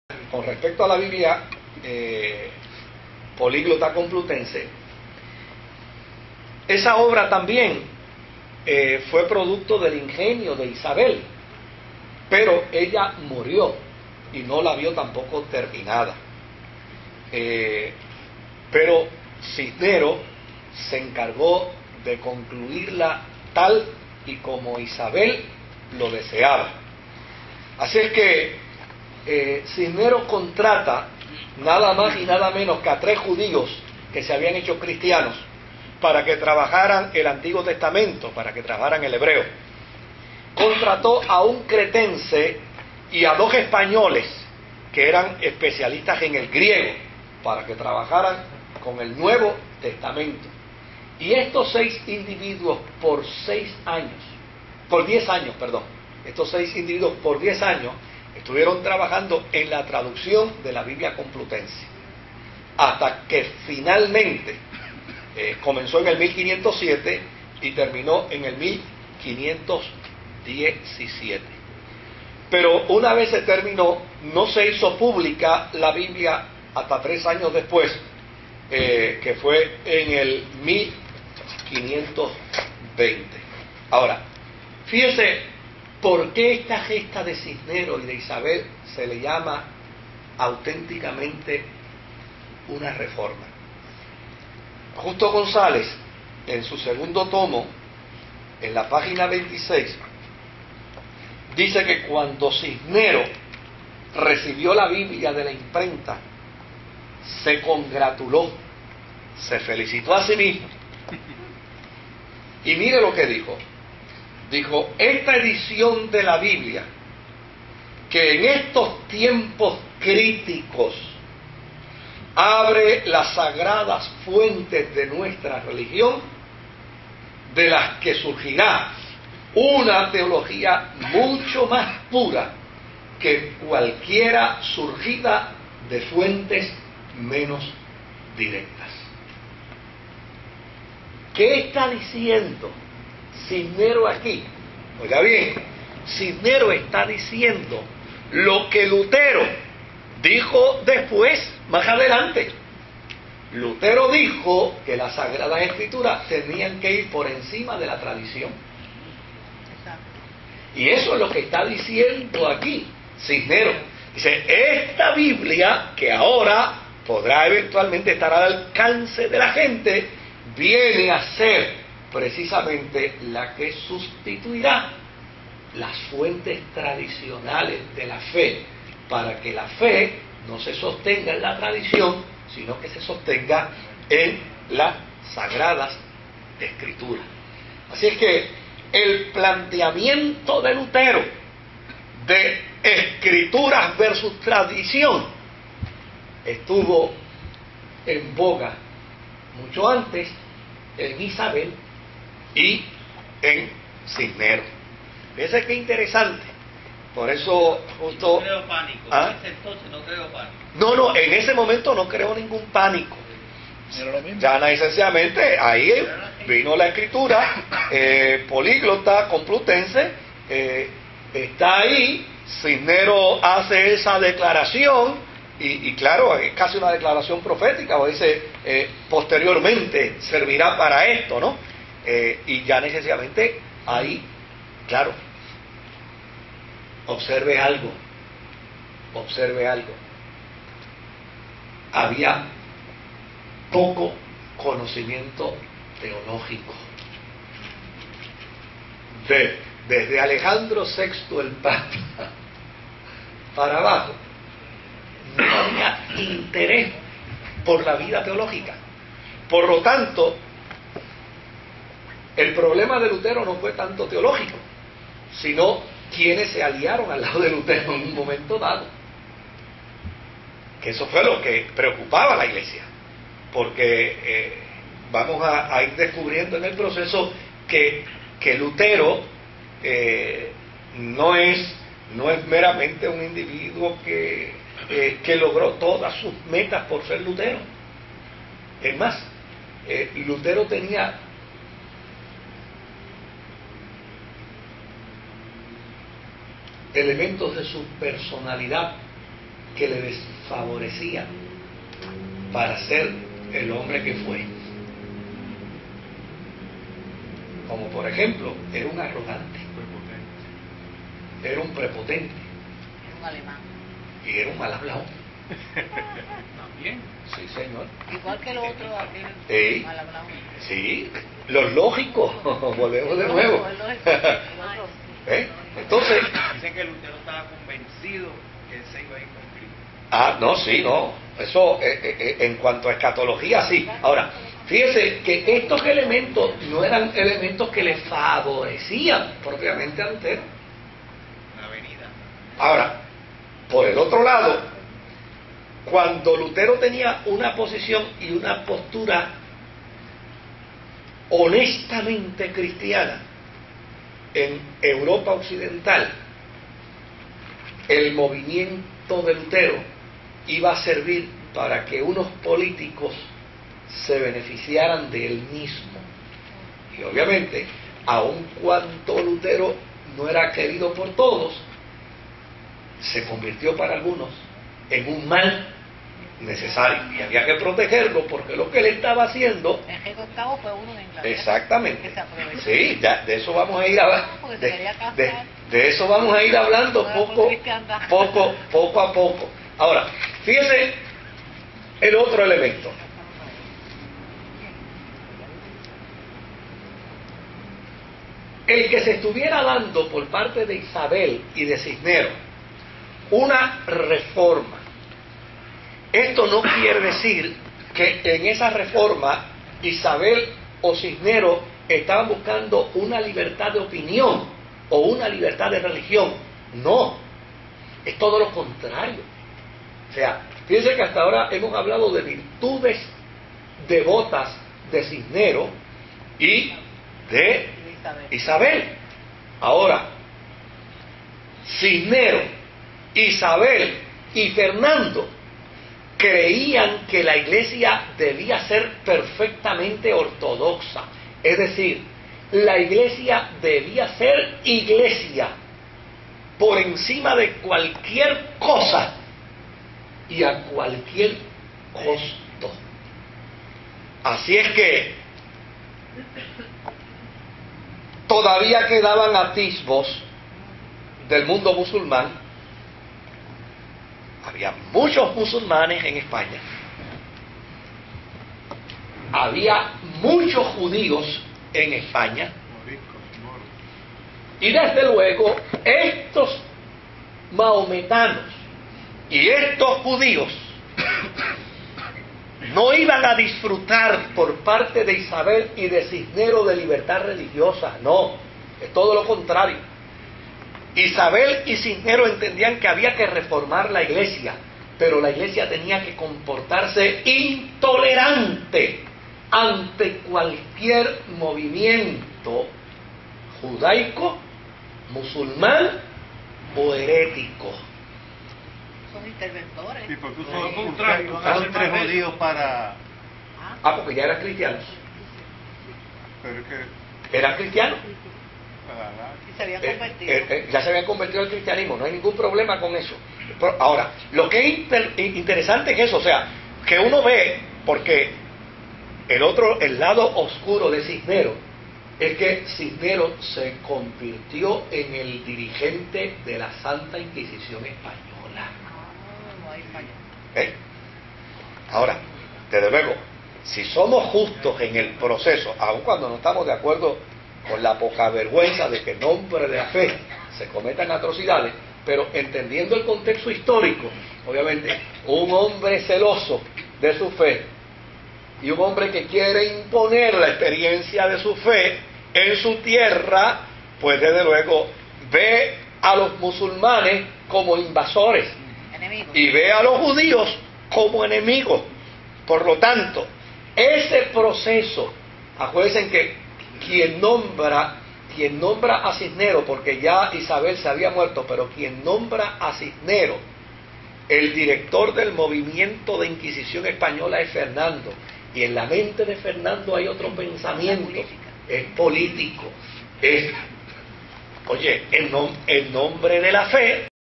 Cristianismo Clase 40 Video (4 d 10) -- Pastor Moisés Román
Estudios Bíblicos por el Pastor Moisés Román Díaz,  de la Iglesia Evangélica Ministerio Sanador de Puerto Rico.  Noviembre 20 de 2007.  Estudio del Cristianismo -- Comenzamos con Martín Lutero
Te exhortamos a escuchar estos estudios  desde: